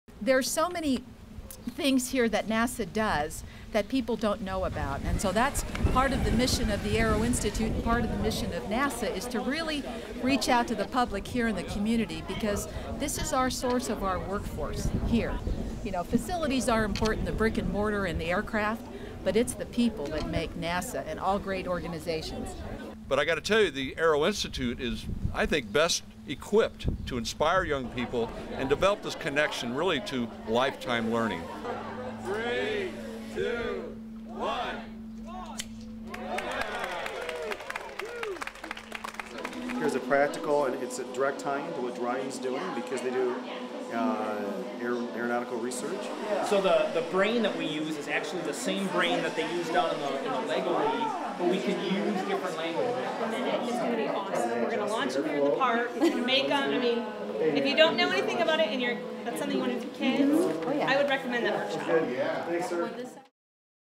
NASA Dryden's Educator and Visitor Centers Reopen
The City of Palmdale, NASA's Dryden Flight Research Center and the AERO Institute recently hosted the reopening of NASA Dryden's Educator Resource and Visitor Centers. Now housed at the AERO Institute in the Palmdale Civic Center, the center has a wealth of NASA educational materials available for teachers, informal educators and parents covering science, technology, engineering and mathematics.